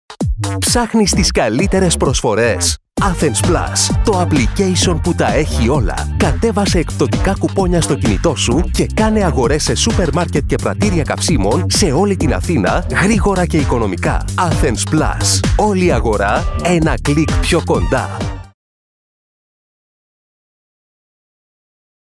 Athens + application tv spot
Commercial spot for Athens+ a phone application that helps you find the best deal around you and delivers the sale coupon in to your phone to claim on the spot!!